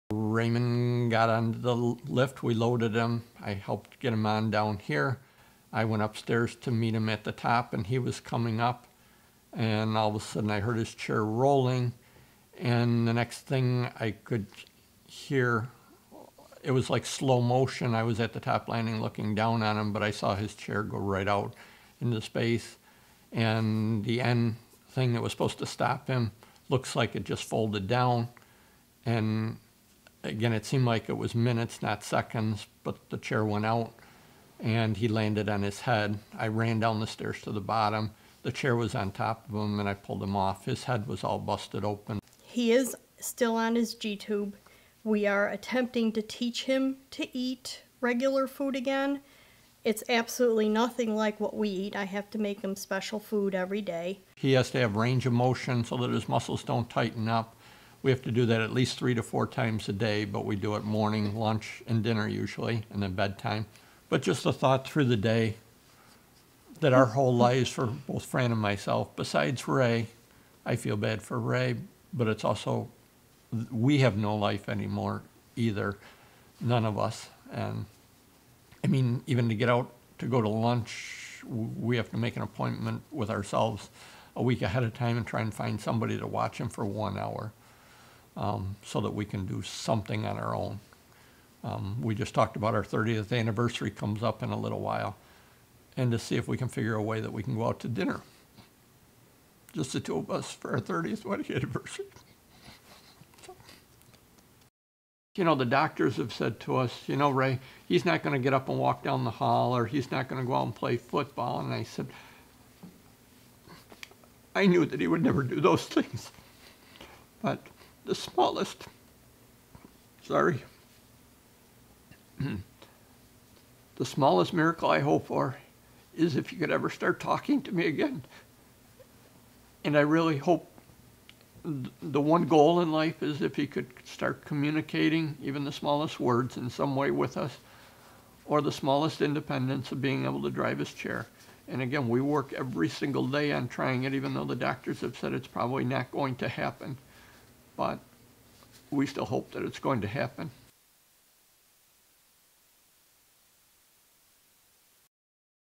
Product injury example
Sample video from a product liability/personal injury case that settled this year (2013). Just Perspective incorporated day in the life footage as the family told their story.  For more information about this Orlando-based legal video company, visit justperspective.com.